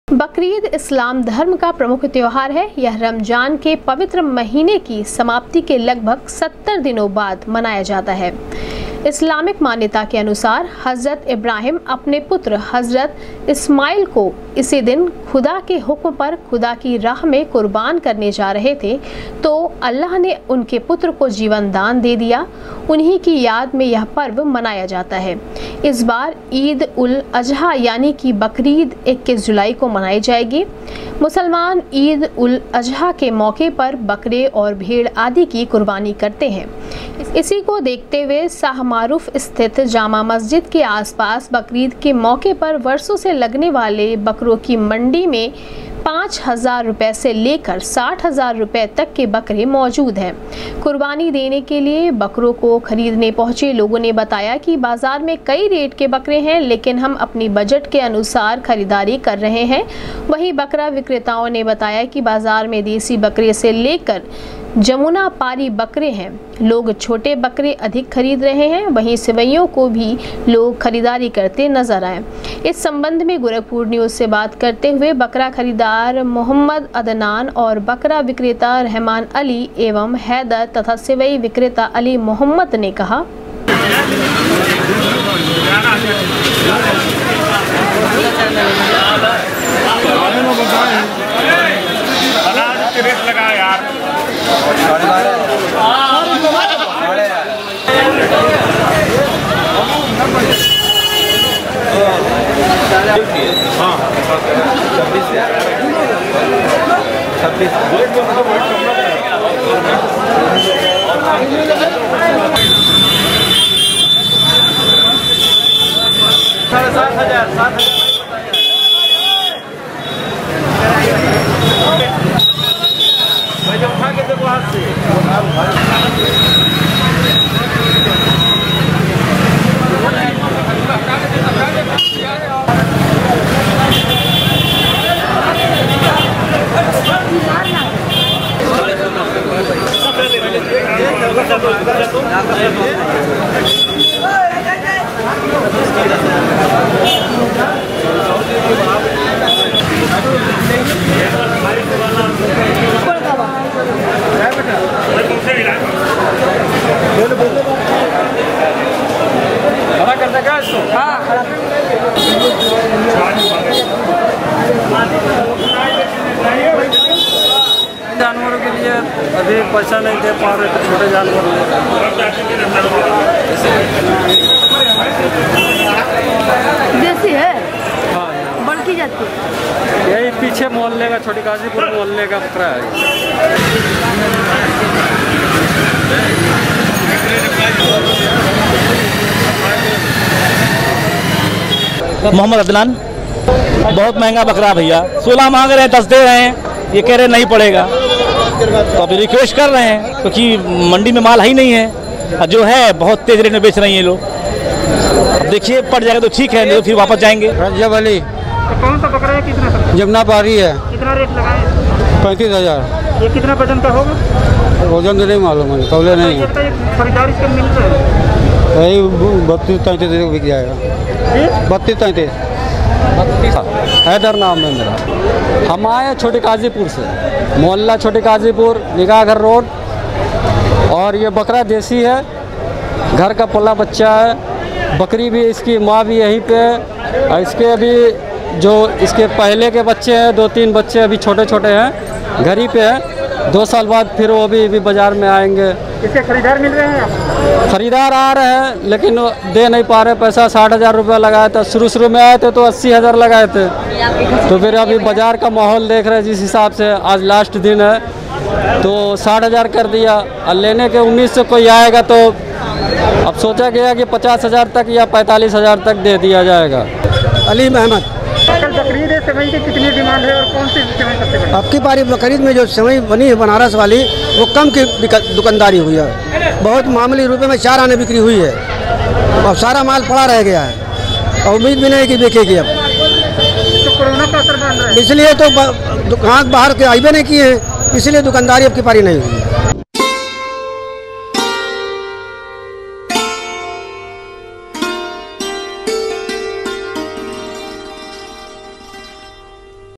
बुधवार को मनाई जाएगी बकरीद, ईद उल अजहा को लेकर बाजारों में रौनक, बाजारों में 05 से 60 हजार
बुधवार को मनाई जाएगी बकरीद, ईद उल अजहा को लेकर बाजारों में रौनक, बाजारों में 05 से 60 हजार रुपए तक बिके बकरे, लोगों ने जमकर की खरीदारी।